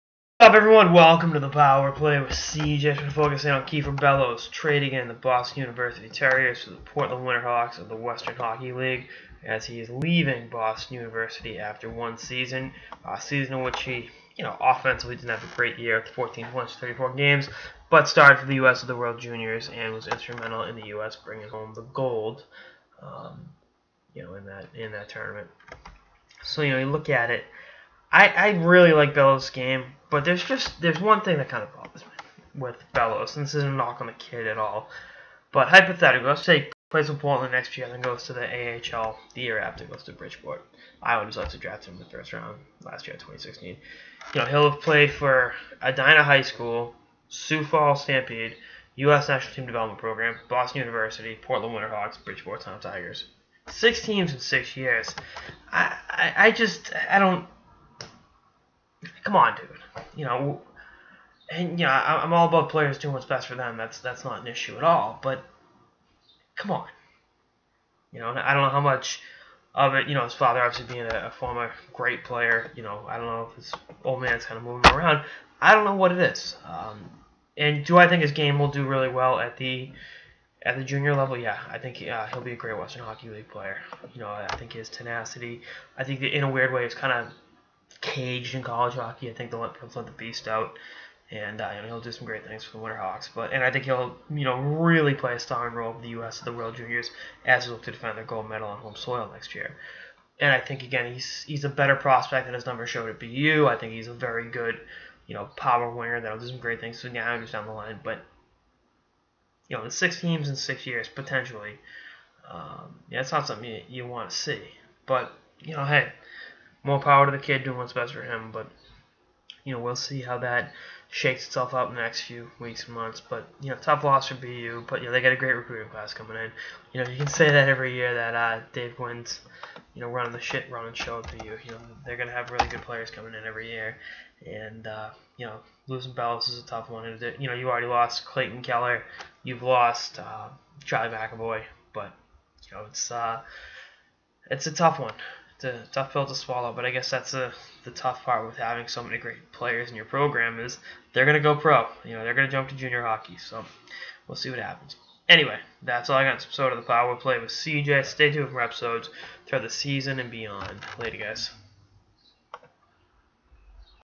Power Play with CJ: Kieffer Bellows to Portland
CJ dishes on the forward going to the WHL and what it means going forward